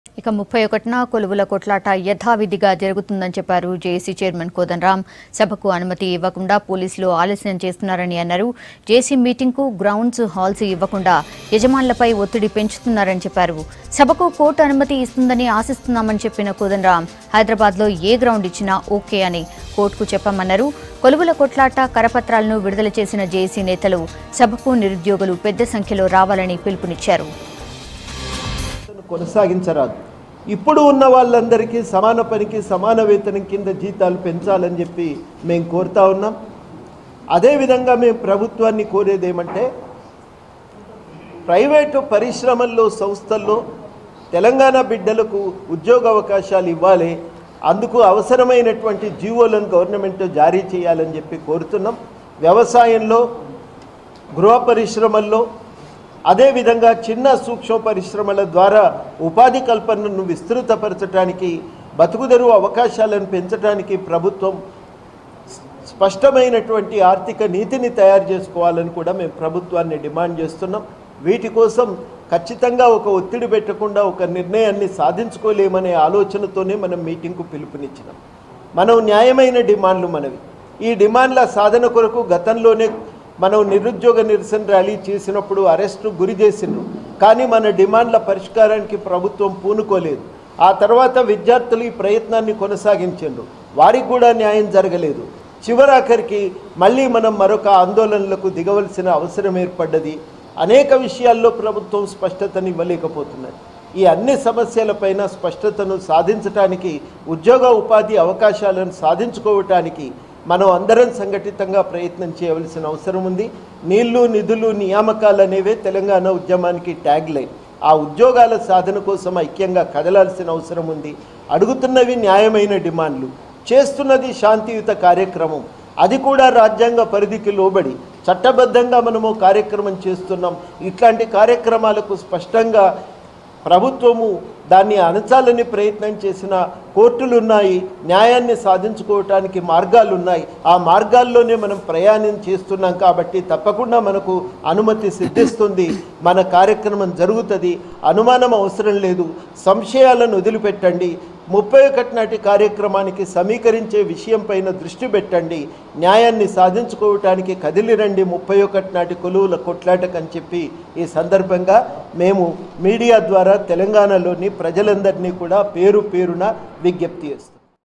JAC Chairman Prof Kodandaram Releases Koluvulakai Kotlaata Program Pamphlets | V6 News
JAC Chairman Prof Kodandaram Releases Koluvulakai Kotlaata Program Pamphlets.